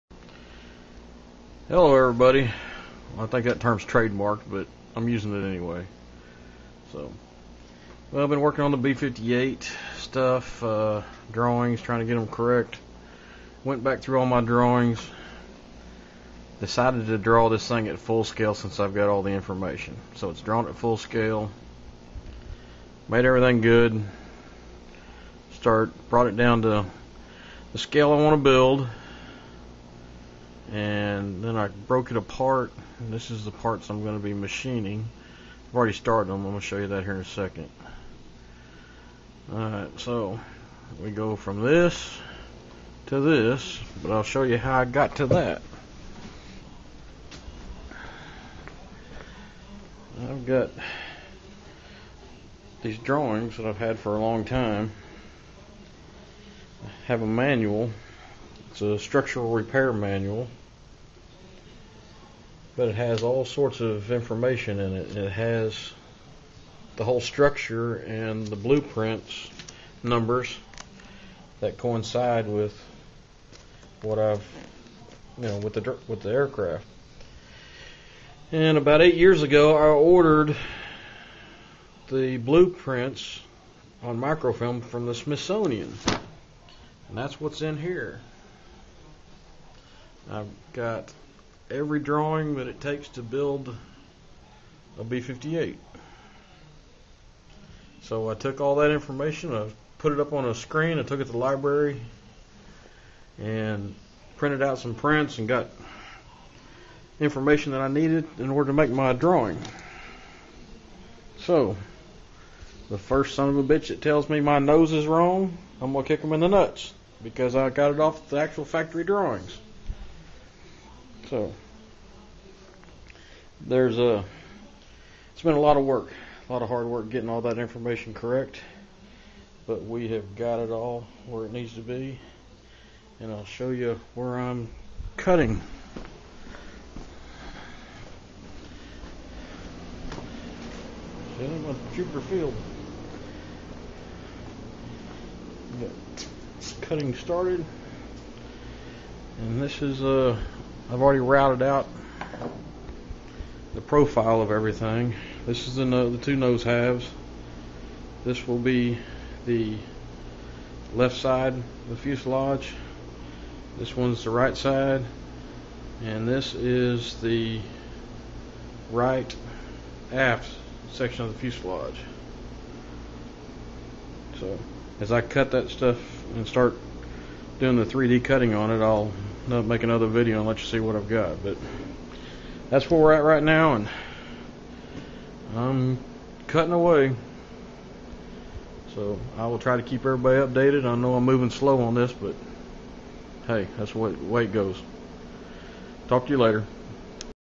Scale B-58 Hustler CNC Start
Starting to CNC cut a new B-58 Hustler at 1/8.8 scale